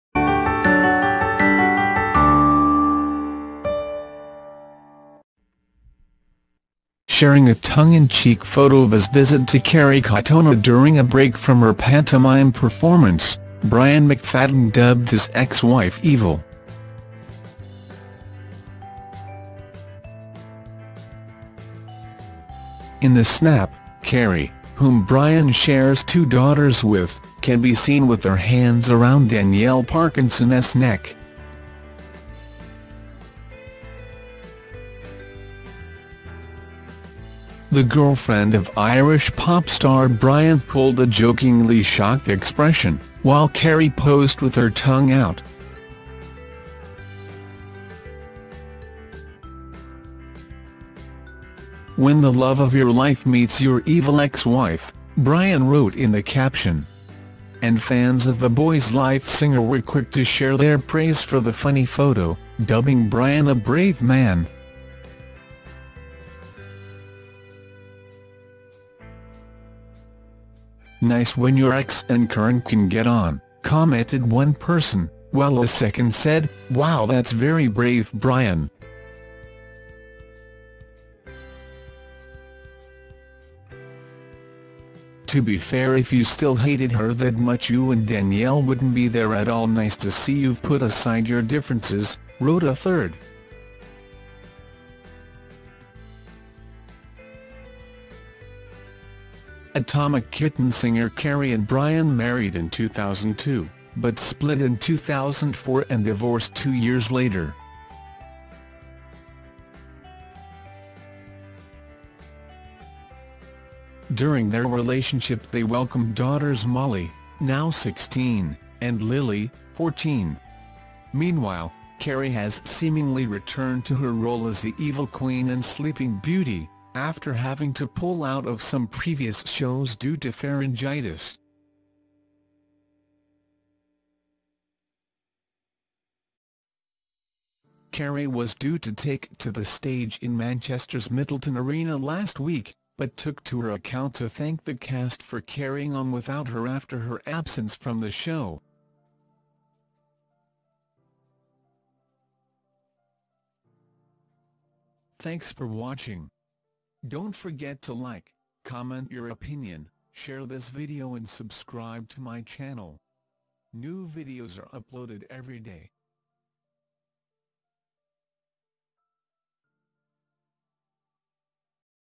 Kerry Katona grabs ex Brian McFaddens woman pal thru the THROAT Sharing a tongue- Kingdom Showbiz
Sharing a tongue-in-cheek image of his go to to Kerry Katona throughout a destroy from her pantomime performance Brian McFadden dubbed his ex wife evil. within the snap Kerry - whom Brian stocks  daughters with - may be visible with her hands around Danielle Parkinson s neck.
Joel Dommett broadcasts hes ENGAGED to woman pal Hannah Cooper  The i am A movie s Kingdom Showbiz: